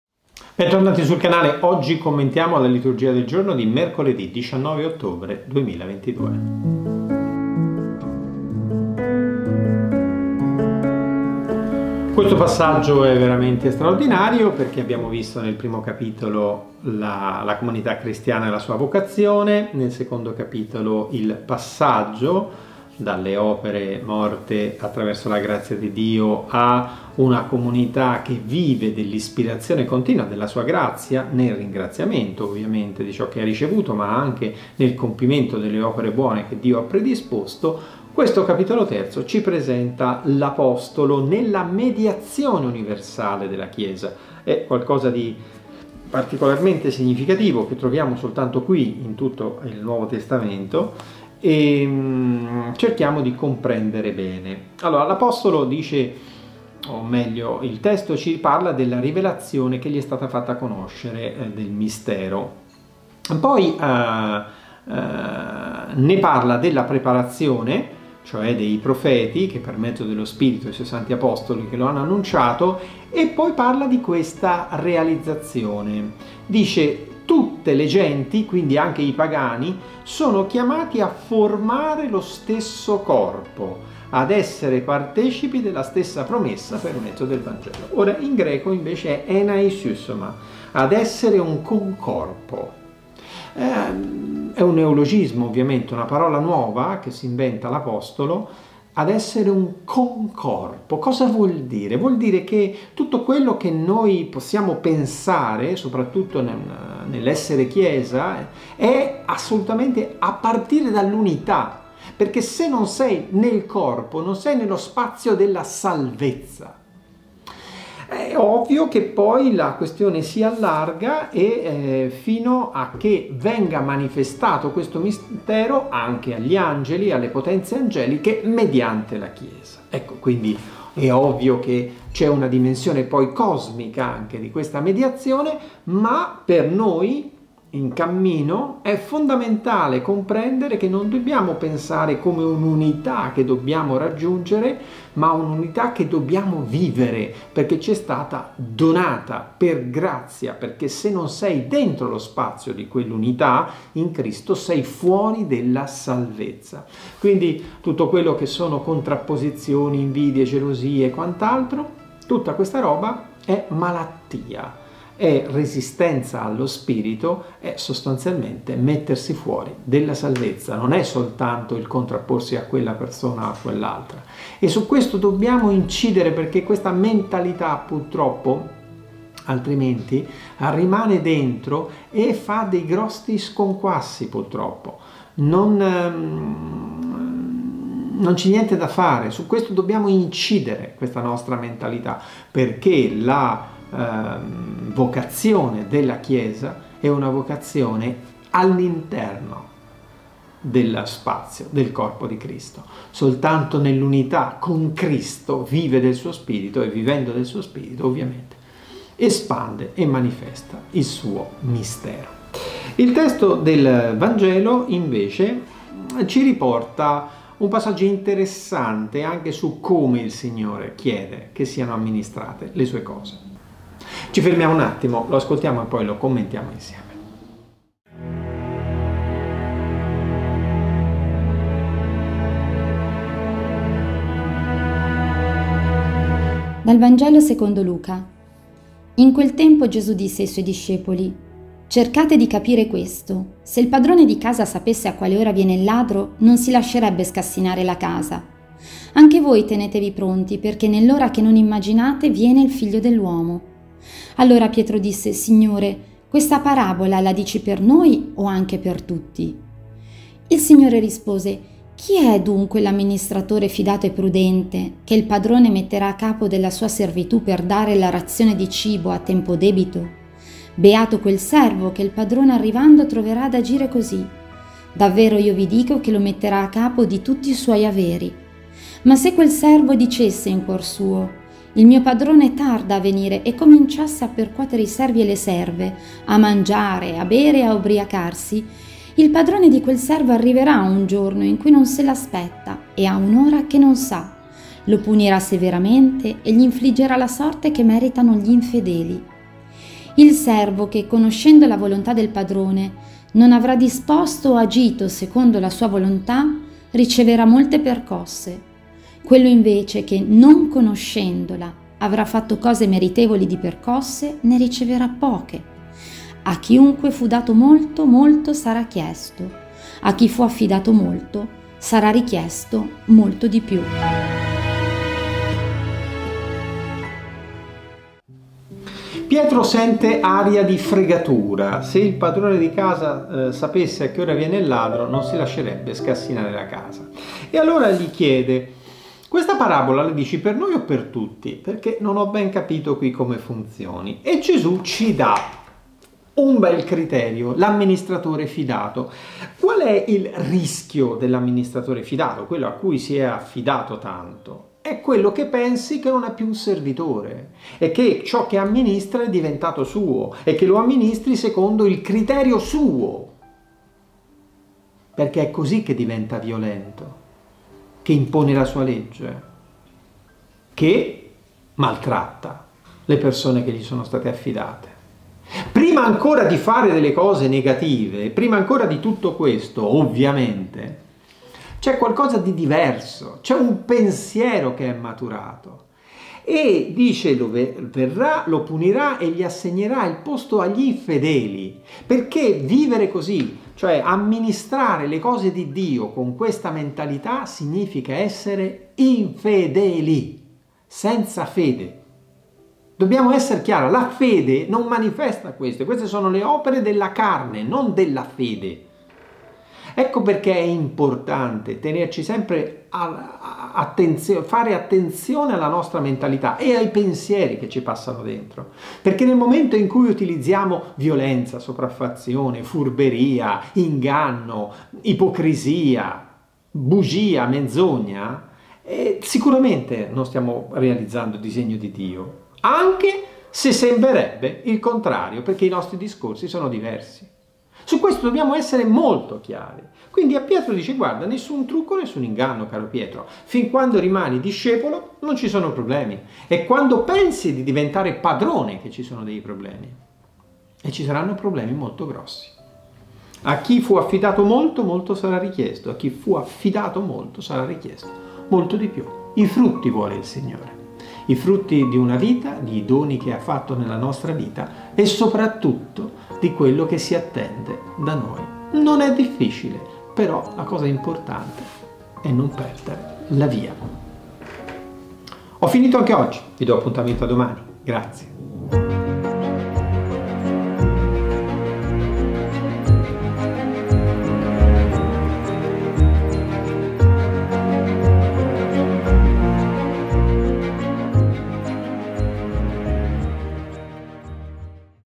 Liturgia del giorno (19 Ottobre 2022)
del giorno 19 OTTOBRE 2022

Ef 3,2-12; Is 12,2-6;
Lc 12,39-48: "A chiunque fu dato molto, molto sarà chiesto; a chi fu affidato molto, sarà richiesto molto di più".